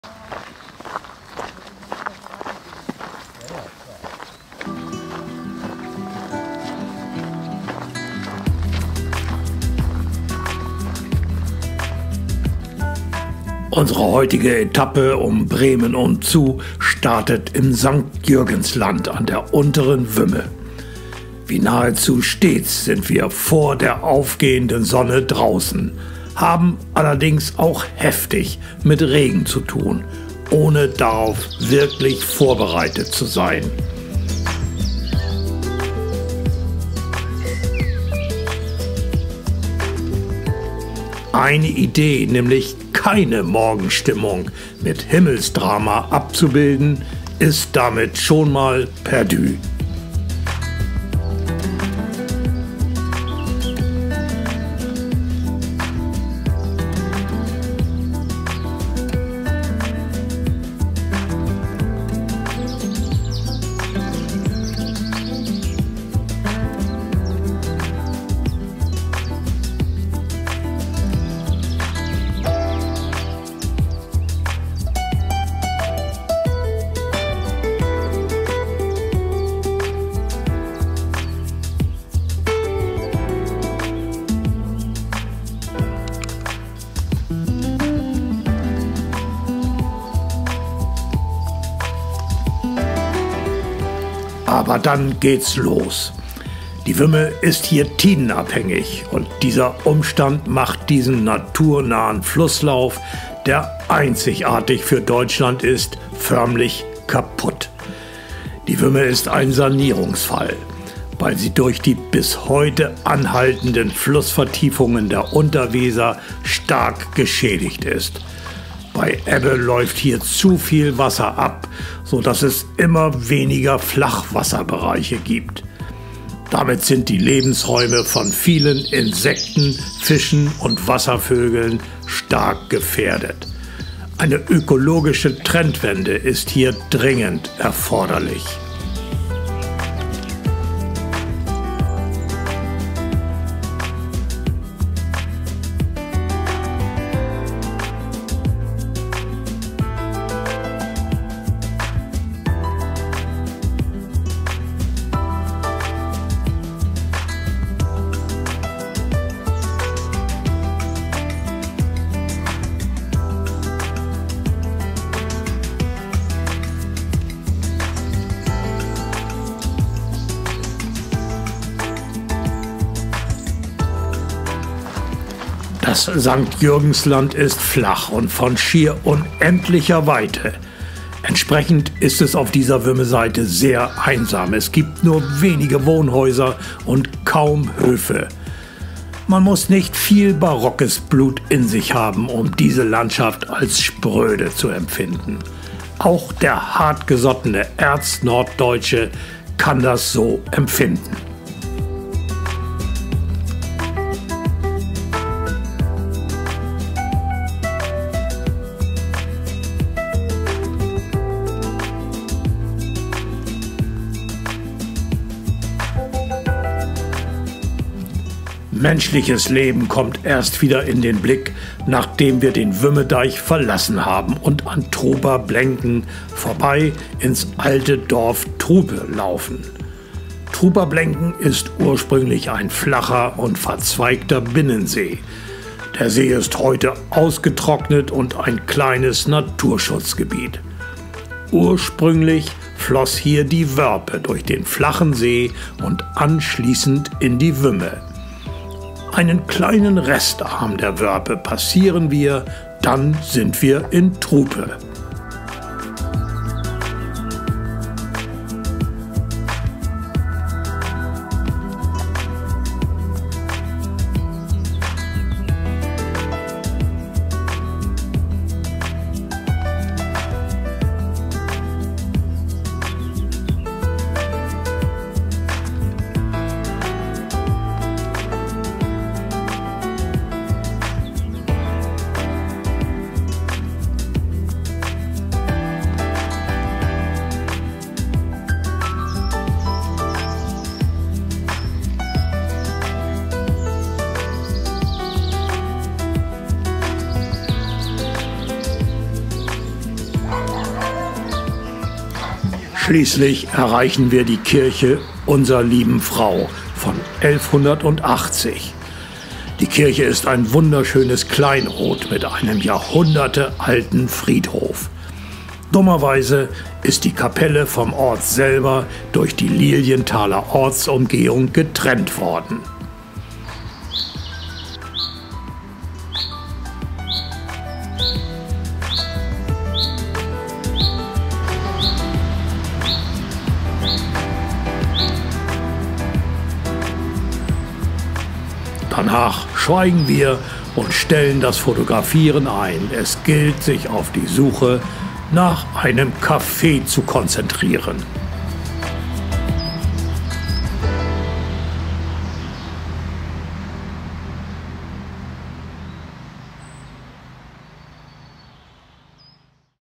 Um Bremen Umzu #04 - vom Wümmeblick nach Lilienthal
Unsere heutige Etappe um Bremen Umzu startet im St. Jürgensland an der Unteren Wümme. Wie nahezu stets sind wir vor der aufgehenden Sonne draußen, haben allerdings auch heftig mit Regen zu tun, ohne darauf wirklich vorbereitet zu sein. Die Wümme ist hier tidenabhängig. Und dieser Umstand macht diesen naturnahen Flusslauf, der einzigartig für Norddeutschland ist, förmlich kaputt. Die Wümme ist ein Sanierungsfall, weil sie durch die bis heute anhaltenden Flussvertiefungen der Unterweser stark geschädigt ist. Bei Ebbe läuft hier zu viel Wasser ab, so dass es immer weniger Flachwasserbereiche gibt. Damit sind die Lebensräume von vielen Insekten, Fischen und Wasservögeln stark gefährdet. Eine ökologische Trendwende ist dringend erforderlich …